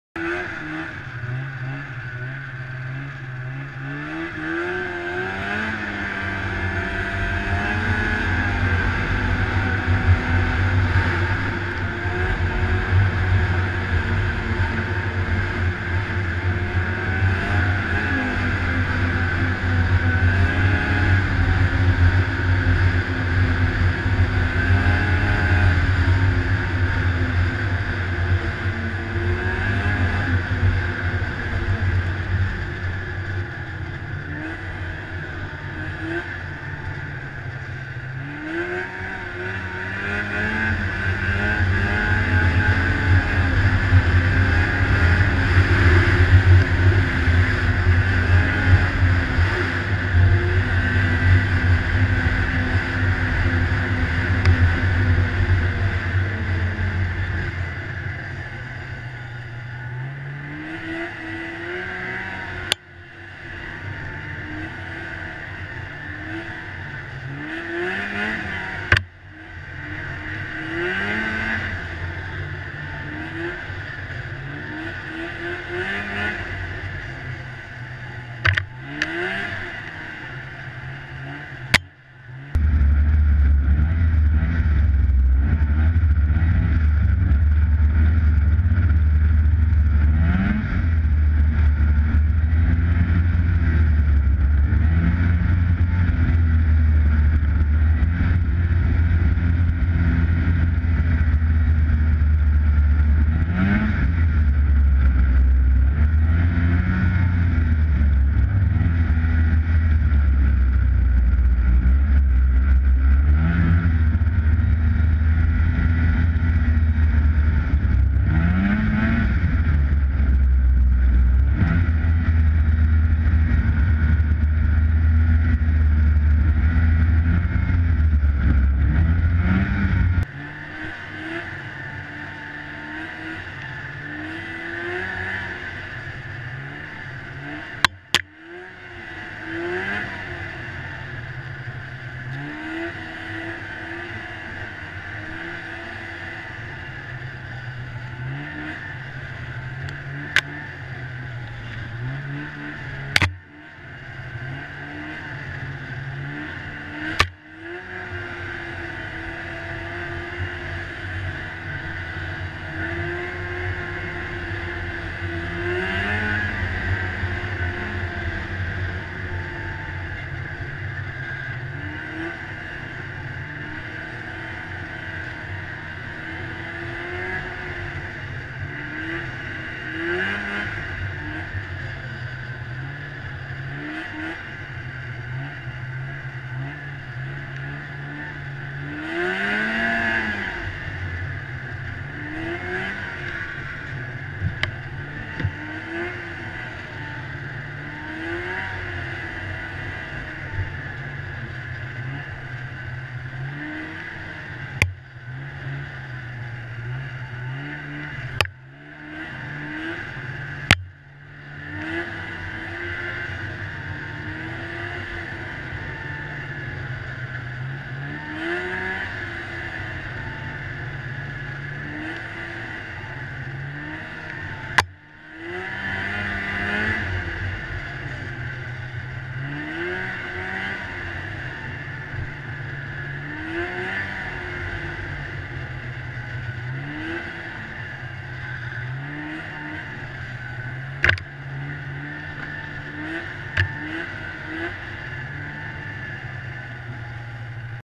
GoPro Hero 3 - Sutton Ontario Trails
Another quick ride threw Sutton West, this time a shorter video, different footage.
Had some uploading issues with this video so the Quality for some devices may not look 1080p. Will try to correct this issue in future uploads!

Footage taken with the GoPro Hero 3.